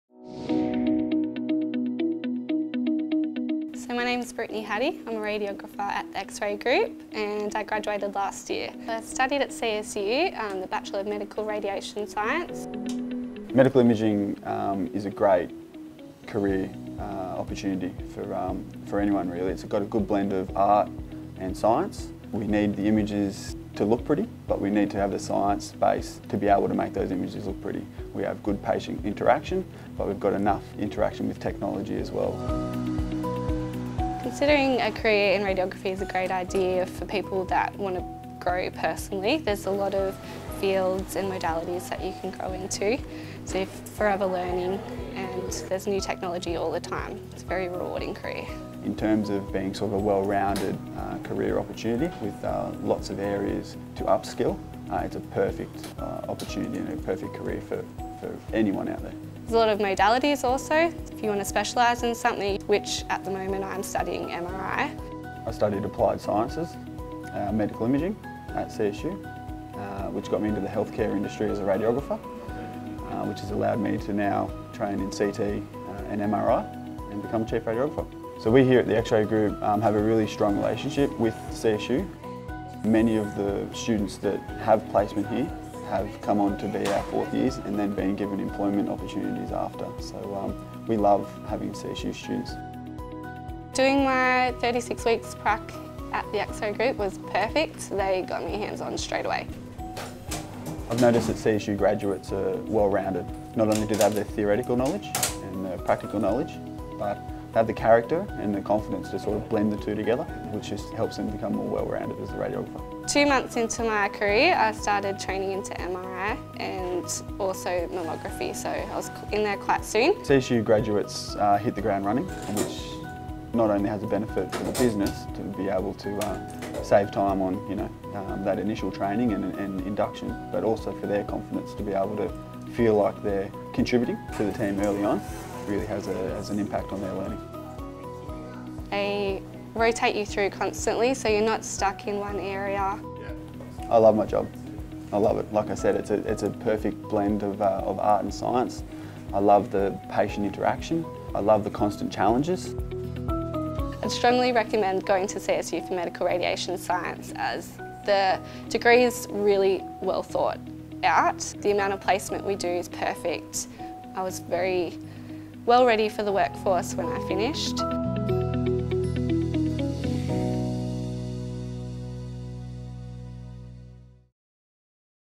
CSU Medical Radiation grads are job ready
Brittany shares her journey of studying at CSU and working within the medical imaging industry while Ty explains why he prefers to employ CSU grads as soon as they graduate.